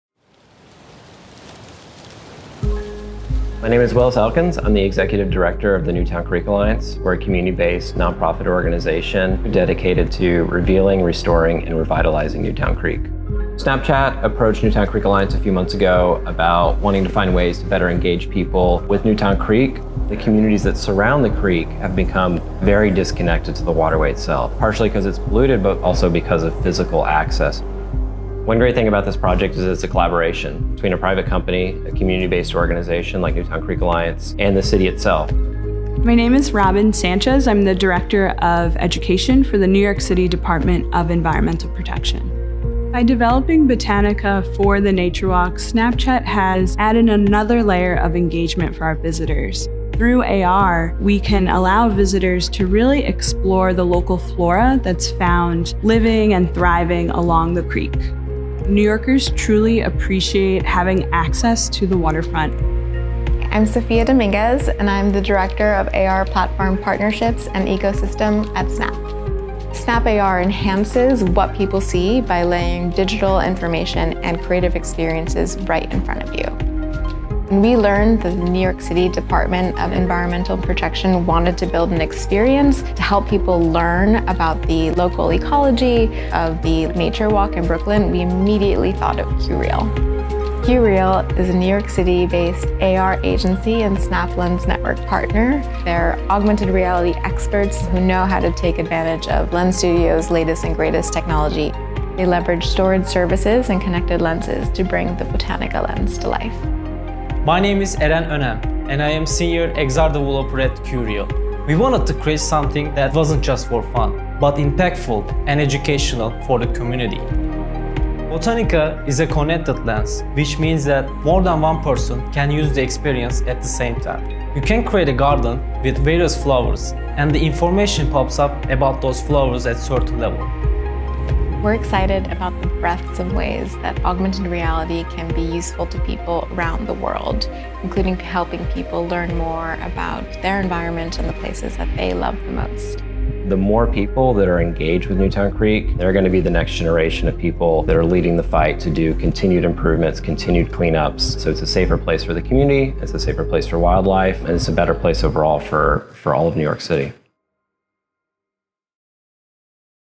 Grow an AR Garden at New York’s Newtown Creek Nature Walk
Augmented reality is grounded in the real world, and now, it can reveal more about what’s planted in the ground too. New Yorkers and visitors alike can now discover more about the world around them through AR while visiting the Newtown Creek Nature Walk, a waterfront promenade richly planted with native trees, shrubs, and flora in Brooklyn’s Greenpoint neighborhood. Botanica is a new AR experience that allows visitors of the Newtown Creek Nature Walk to learn about more than 25 native flowers and plants found in the local ecosystem by growing an AR garden with friends.